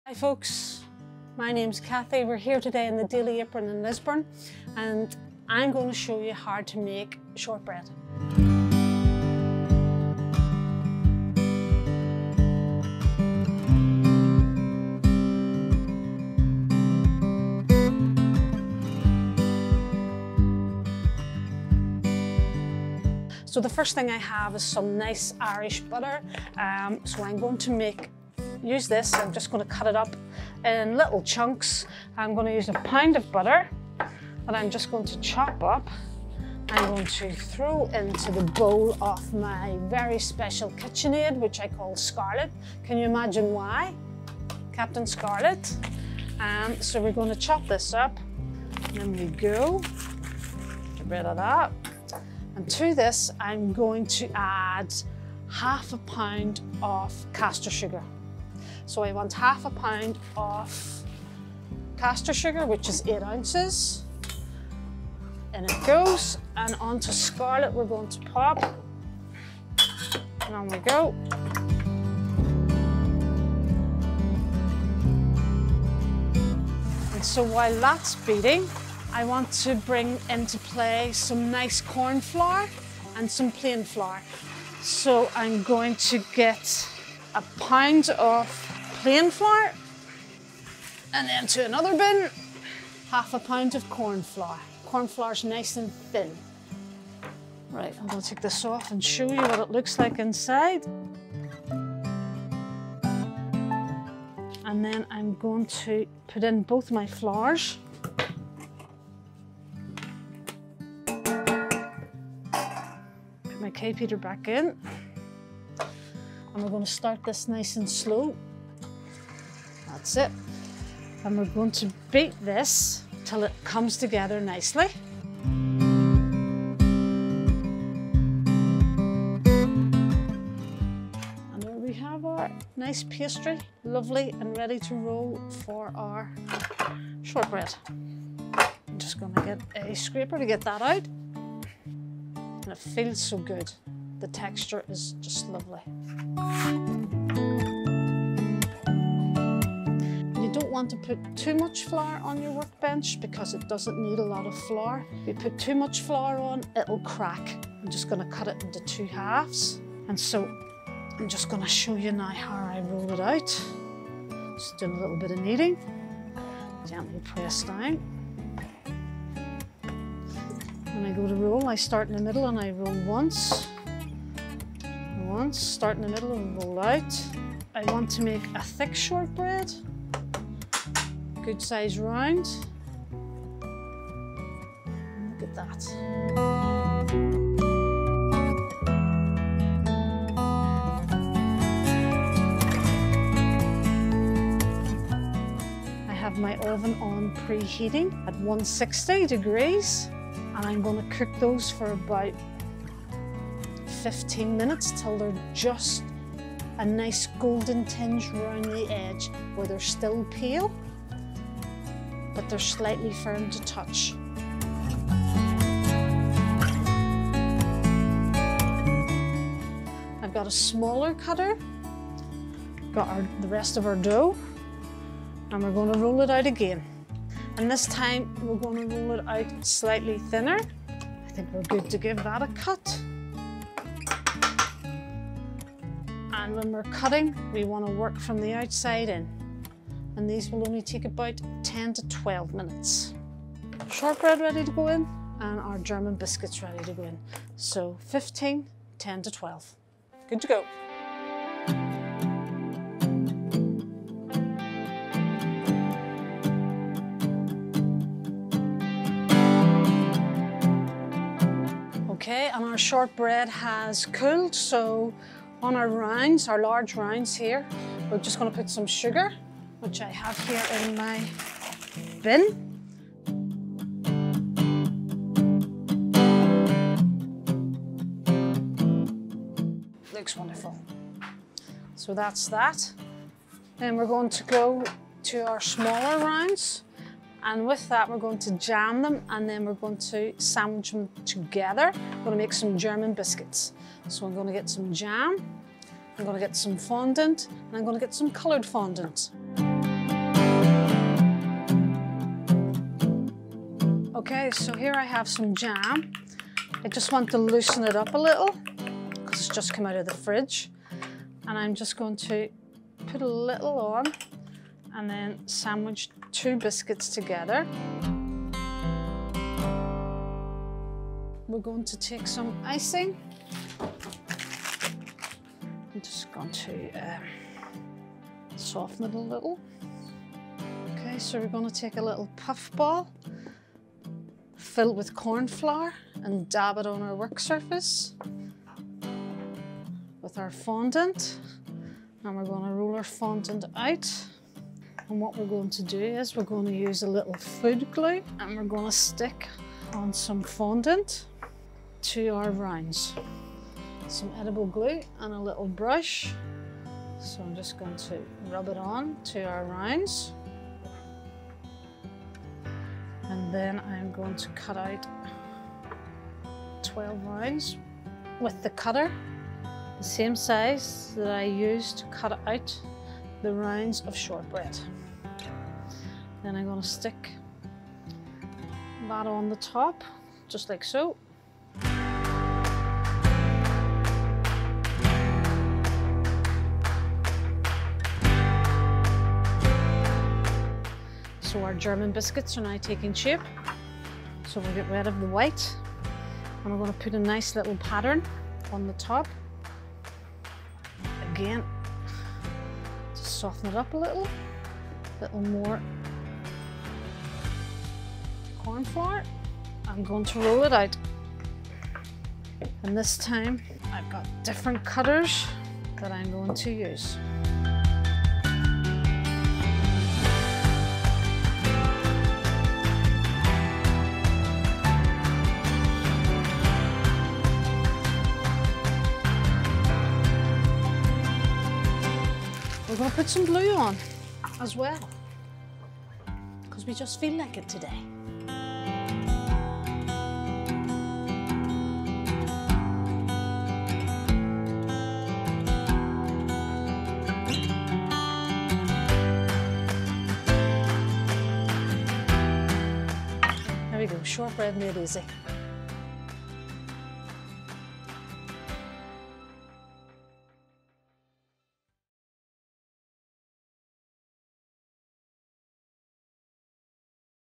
How to Make Shortbread With The Daily Apron | The Recipe Book | Shortbread Tutorial | Biscuits
Have you ever wanted to know how to make shortbread biscuits?

In our video, Kathy from The Daily Apron, a fantastic cafe, and bakery in Lisburn, Northern Ireland will walk you through this step-by-step guide on how to make shortbread at home.

For the ingredients, you will need:

- 1lb of butter
- 1/2lb of caster sugar
- 1lb of plain flour
- 1/2lb of corn flour

Additional Ingredients for German Biscuits:
- Jam (Strawberry)
- Fondant for decoration

This is a great and easy baking tutorial perfect for beginners to home baking looking to create their very own shortbread biscuits as well as german biscuits.

Thank you to The Daily Apron for sharing their delicious recipes with us and for more information and to try the amazing bakes themselves, be sure to check out their Facebook page!